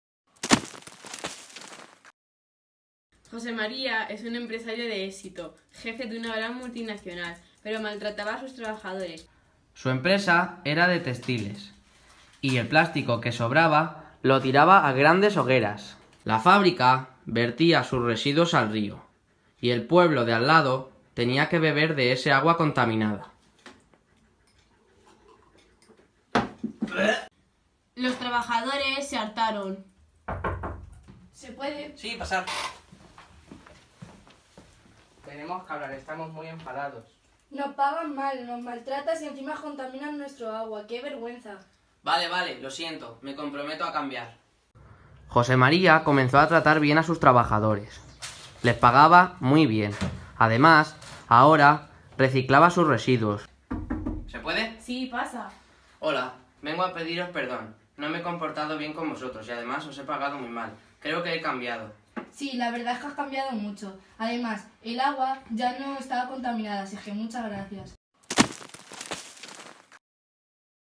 La industria de José María
Video donde se muestra cómo un empresario se da cuenta de su error al tratar mal la creación y logra cambiar.

COLEGIO DIVINA PASTORA 4º ESO
Festival de Clipmetrajes de Manos Unidas